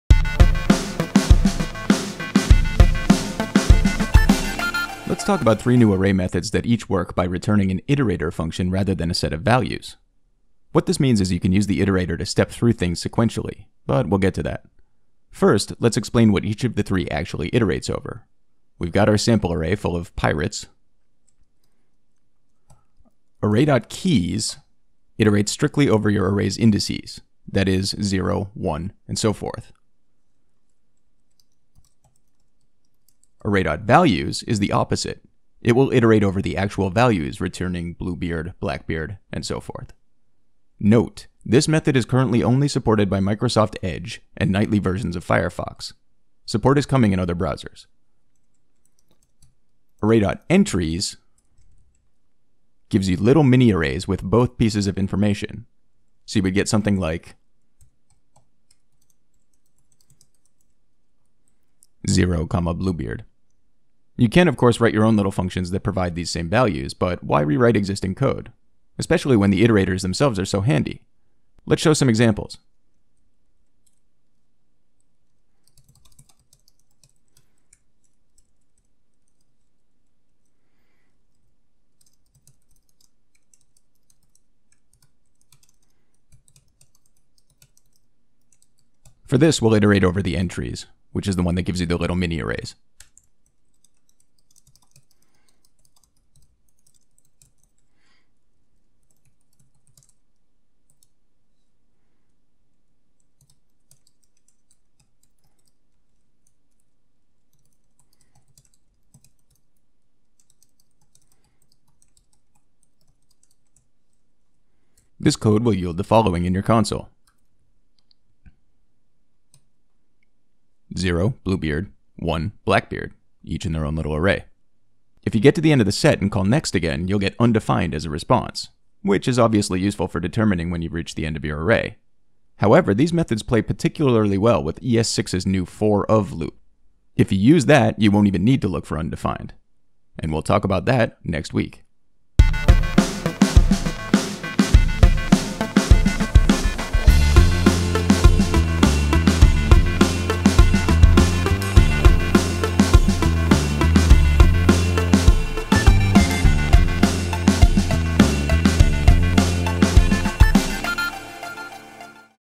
JS Quick Hits - Array Iterators
ES 2015 brings us some new, really useful iterator methods. Three, in fact. Let's check them out!